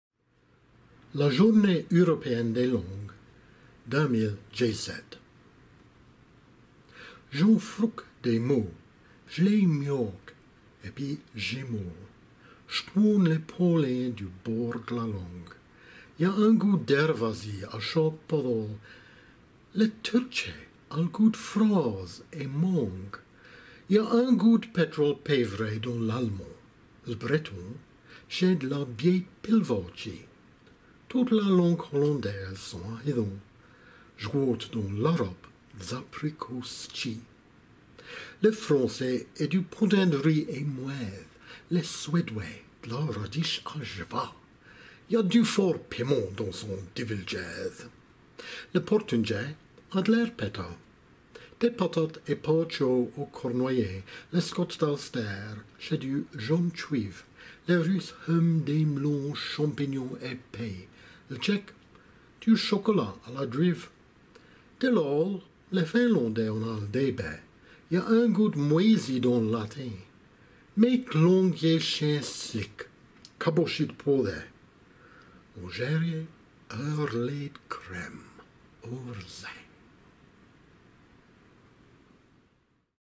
La Journée Ûropéenne des Langues 2017
J'enfrouque des mots, j'les miâque épis gînmole;
j'touônne lé pâlîn du bord d'la langue.
Y'a un goût d'èrvas-y à châque pathole:
lé Turtchais a l'goût d'frâses et mangues;
y'a un goût d'pétrole paîvrée dans l'Allemand;
l'Bréton, ch'est d'la bliête pilvâtchie;
toute la langue Hollandaise sent à héthan;
j'gouôte dans l'Arabe, d's apricots s'tchis.
Lé Français est du podîn d'riz ès mouaithes;
lé Suédouais - d'la radiche à j'va,
y'a du fort pînment dans san divuldgéthe;
lé Portûndgais a d'l'èrpéta.
Des patates et pâtchieaux au Cornouaillais;
lé Scots d'Ulstèr, ch'est du jaune tchuivre;
lé Russe heunme des m'lons, champîngnons et pais;
l'Tchèque, du chocolat à la d'rive.
Dé l'âl? Lé Fînlandais en a l'dêbet;
y'a un goût d'mouaîsi dans l'Latîn;
mais qu'l'Angliaîchîn s'lique, cabochi d'pouothé...
Au Jèrriais? Un r'lais d'crème au r'sîn.

Geraint Jennings